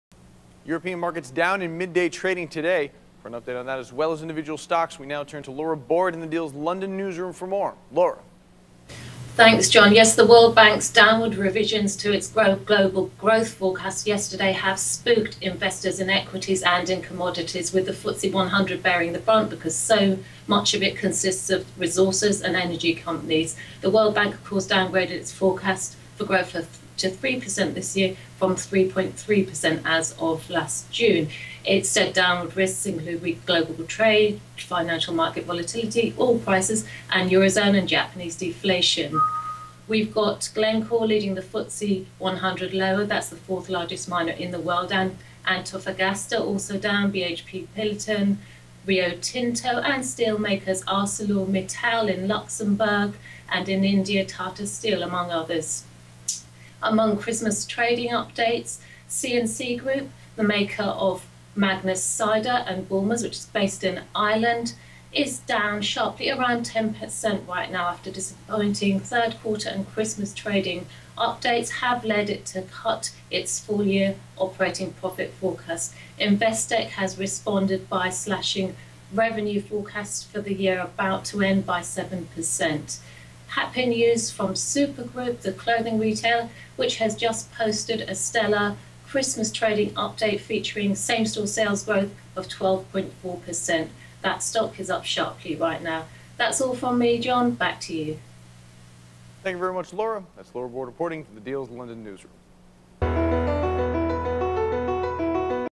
Glencore, Antofagasta, Rio Tinto and BHP Post Steep Losses, Weighing on the FTSE 100
European stock indices decline, led by resources companies and FTSE 100, after the World Bank cut its global growth forecast for this year and next. Predicting growth of 3%, instead of the 3.4% it forecast in June, it cited weak global trade among the risks. Losses among resources companies including Glencore and Antofagasta, steelmakers including ArcelorMittal and oil groups like BP suppress enthusiasm generated by a EU court development seen likely to facilitate the ECB’s expected launch of a program of government bond buying. Meanwhile, in London, Christmas trading updates in the consumer sector continue, weighing on drinks group C&C but lifting clothing retailer SuperGroup.